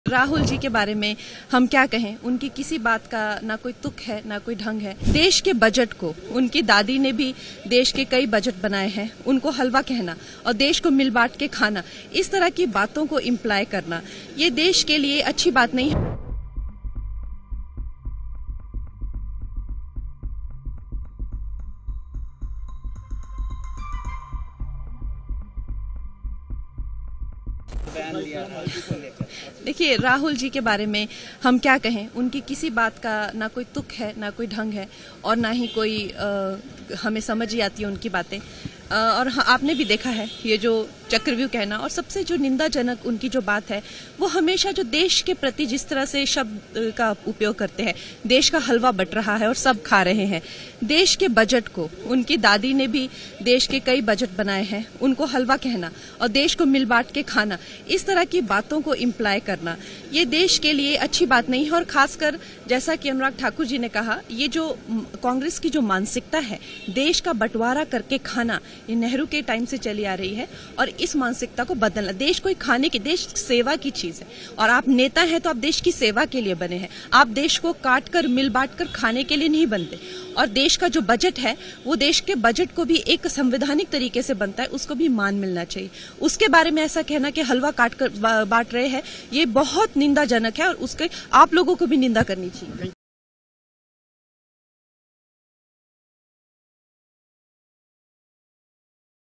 Kangana Ranaut Criticizes Rahul Gandhi's Remarks on Nation
Kangana On Rahul Gandhi: BJP MP Kangana Ranaut criticized Rahul Gandhi, stating, "He makes no sense, at least I don't get what he says." Kangana Ranaut condemned his language about the country, saying it was harmful. Echoing Anurag Thakur's sentiments, Kangana Ranaut added, "Congress' mentality is to break the nation in pieces to benefit themselves, this thing has been going on since the time of Pandit Jawaharlal Nehru."